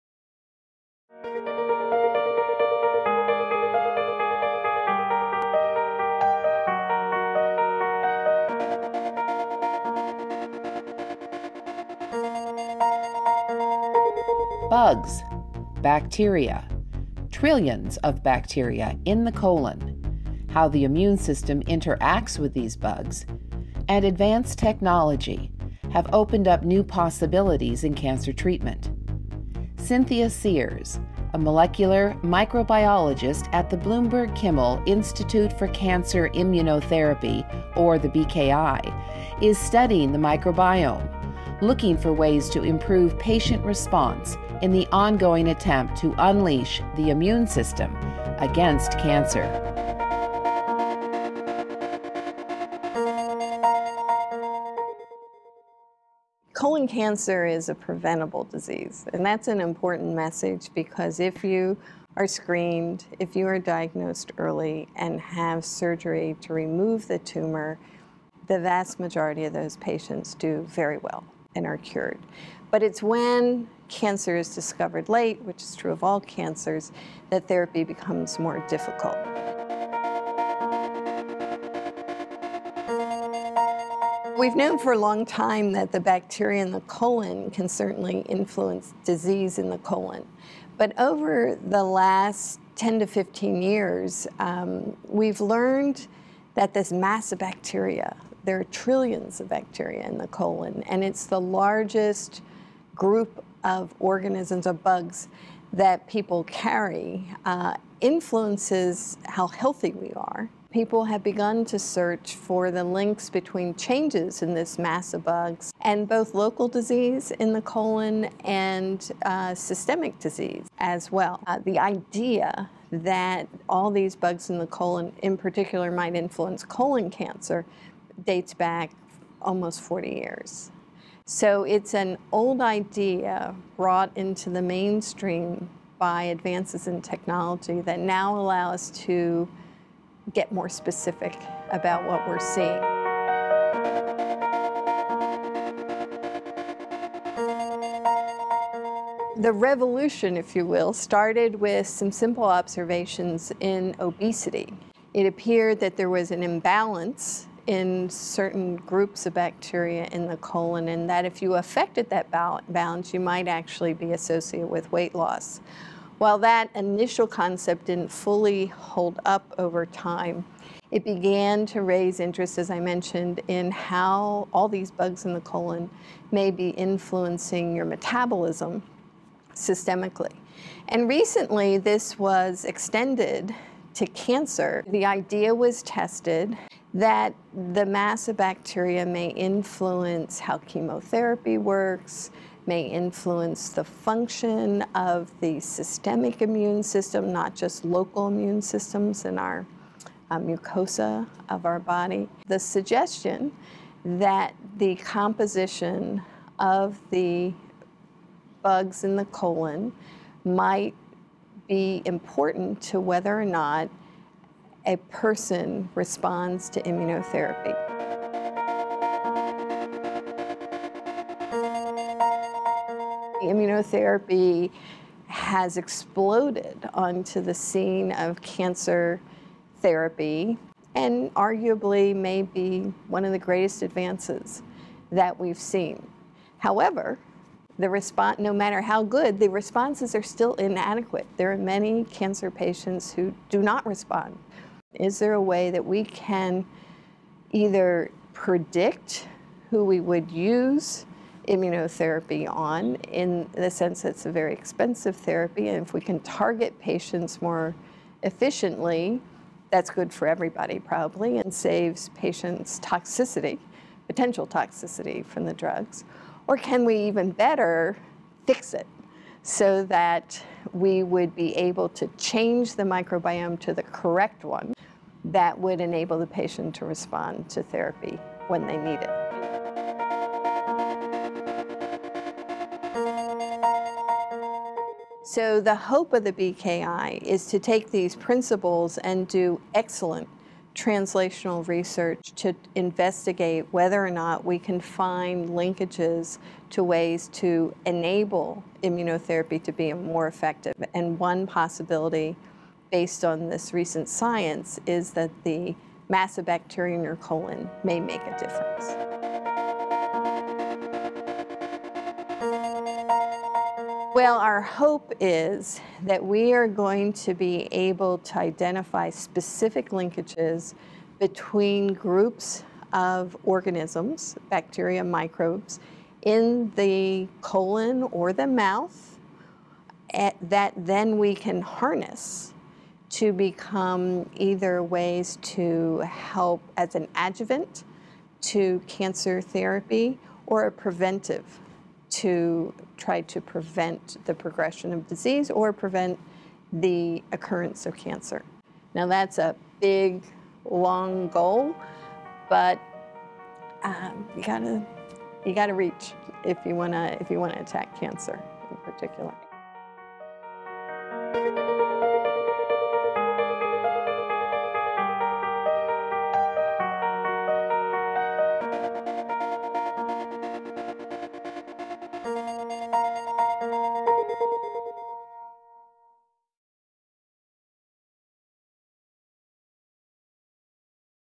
Immunotherapy and the Microbiome
Cynthia Sears, M.D., a molecular microbiologist at The Bloomberg~Kimmel Institute for Cancer Immunotherapy is studying the microbiome, looking for ways to improve patients response in the ongoing attempt to unleash the immune system against cancer.